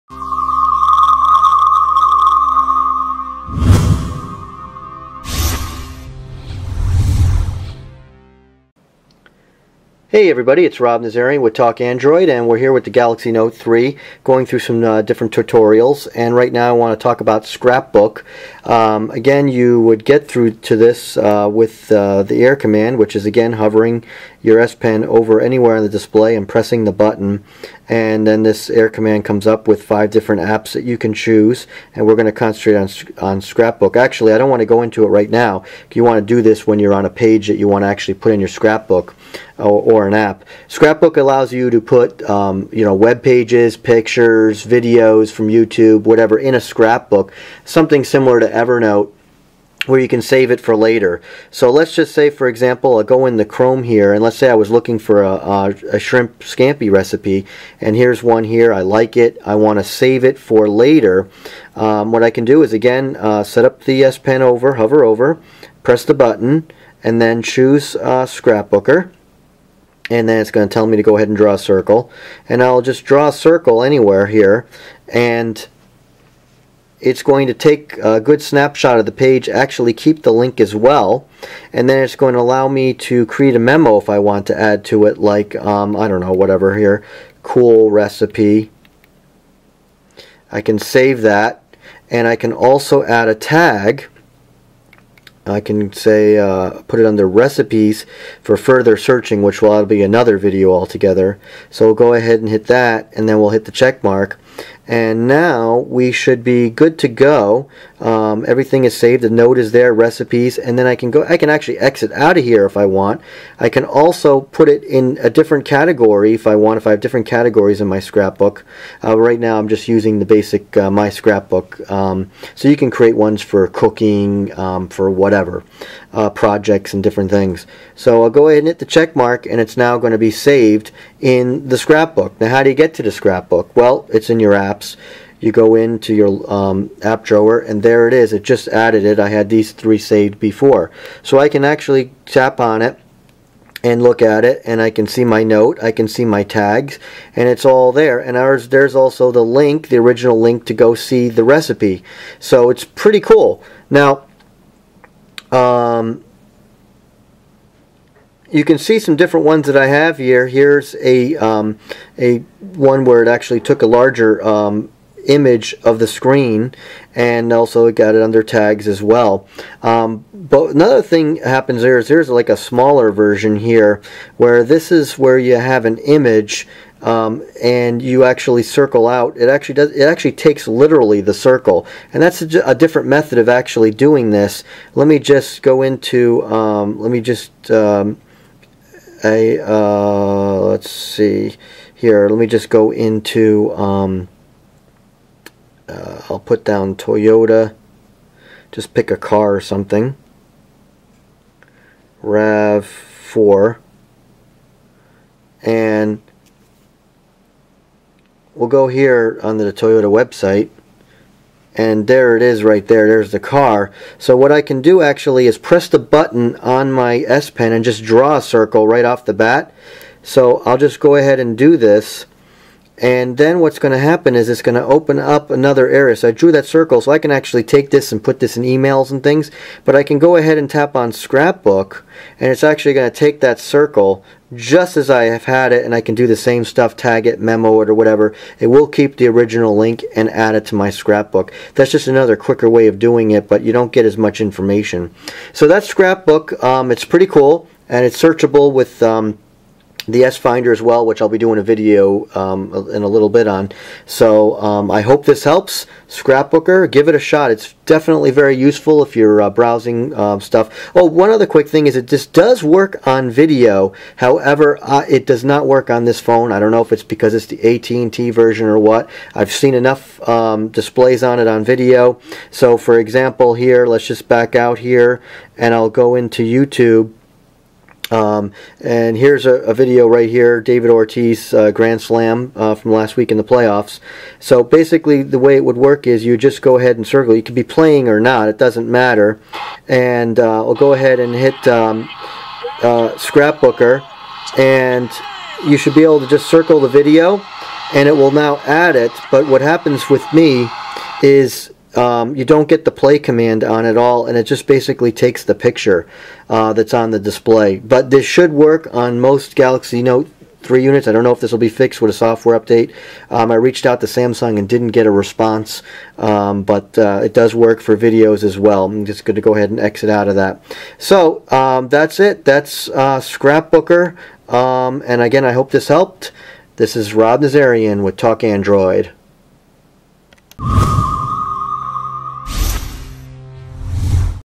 How to use Scrap Book on the Samsung Galaxy Note 3
Samsung has a new feature that debuted on the Galaxy Note 3, and it's called Scrapbook. It's actually quite useful. It allows you to save content into "Scrapbooks." This can be a screenshot from an app, a Web page, or a YouTube video. When you grab a Web page, the link is saved along with it for future reference. You can also categorize your scrapbook with different folders and add tags and/or Notes to each item you have clipped.

To add something to your Scrapbook, just open Air Command by hovering your S Pen over the display and pressing the button on the S Pen. Choose the second option, Scrapbooker. Then draw a circle around the contents you want to save. You will then be given the opportunity to add tags and/or a Note as well as choose the folder you would like to add it to. When you are all set, just tap on the checkmark at the top right to save it. There is also an option to add an item to your scrapbook bypassing Air Command. Just press and hold the S Pen button and draw a circle on the screen and select Scrapbook. For whatever reason, it doesn't grab as much information, but it will still capture the URL link. Lastly, you can sync Scrapbook to your Samsung account so it's accessible on other devices.